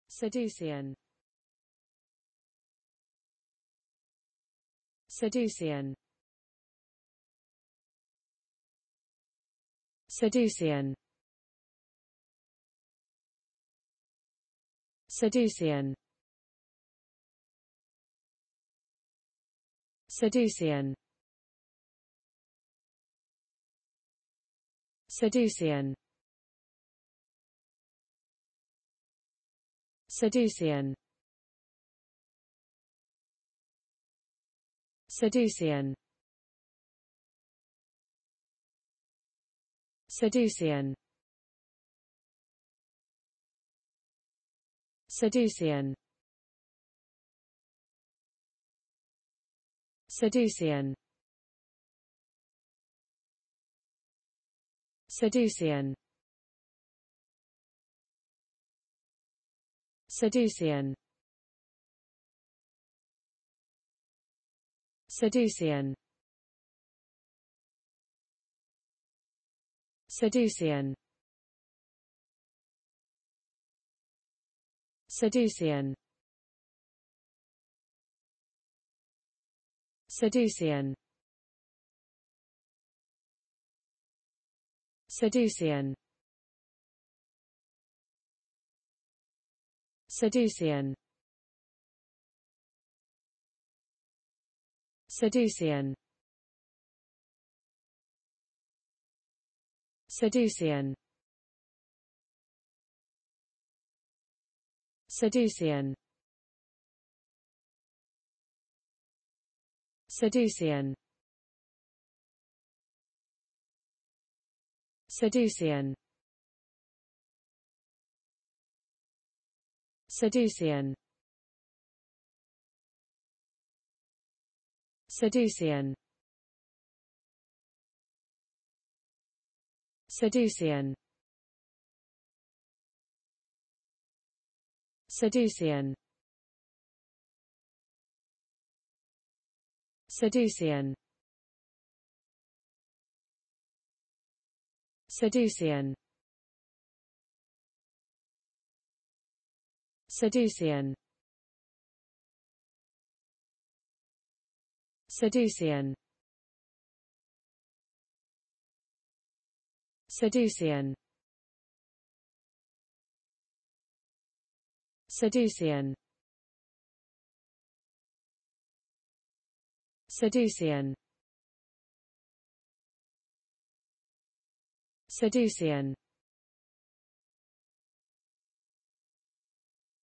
Sadducean. How to pronounce the english word Sadducean .Start with S. Learn from me.
Follow me!Learn from me how to pronounce the english word Sadducean.This video shows you how to pronounce the English word Sadducean .